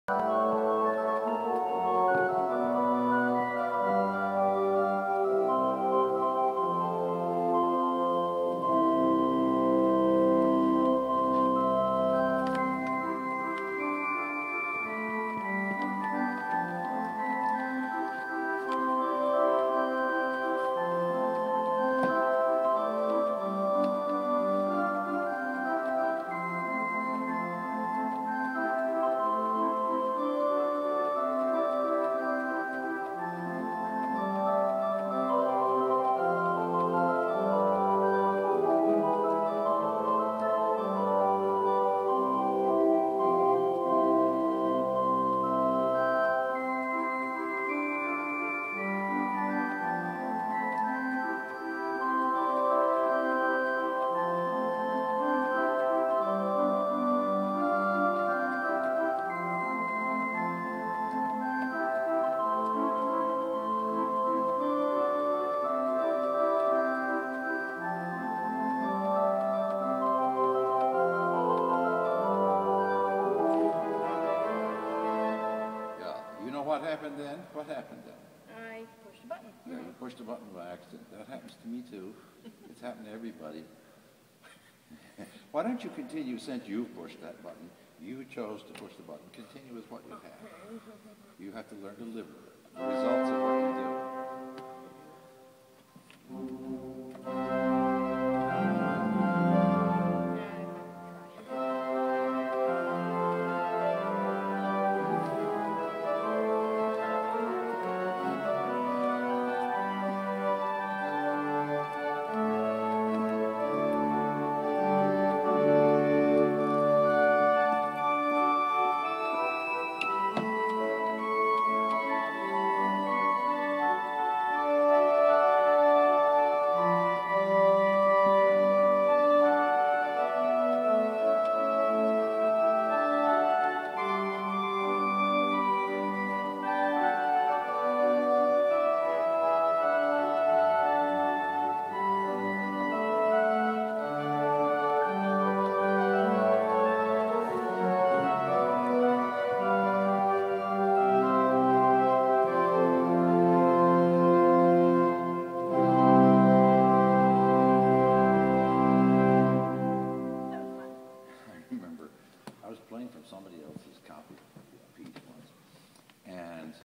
Thomas Murray and Thomas Valenti
Thomas Valenti plays world famous organ at Woosley Hall at Yale.